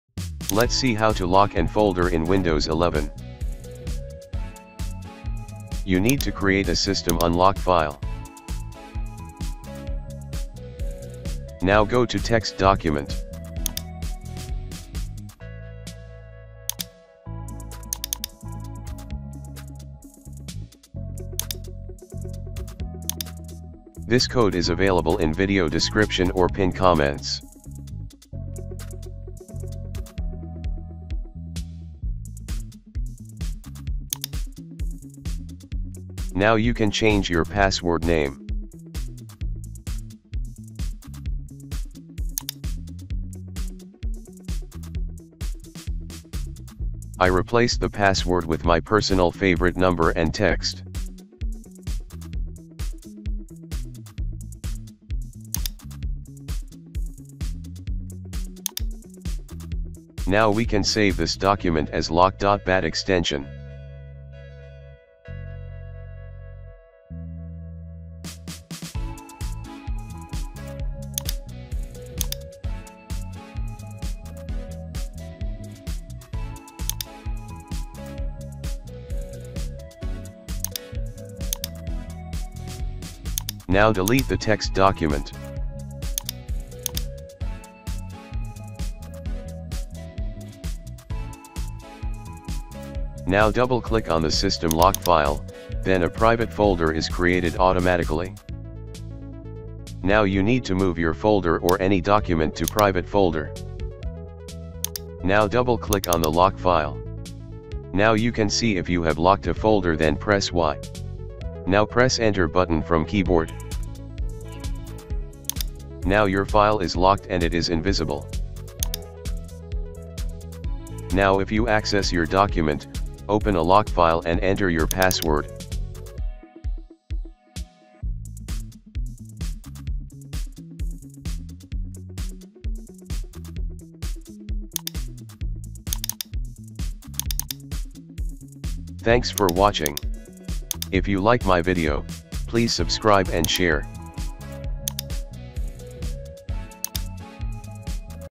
How to lock a folder in windows 11 without software
Let's see how to lock folders in Windows 11 without any software

Code .In Pin Comment .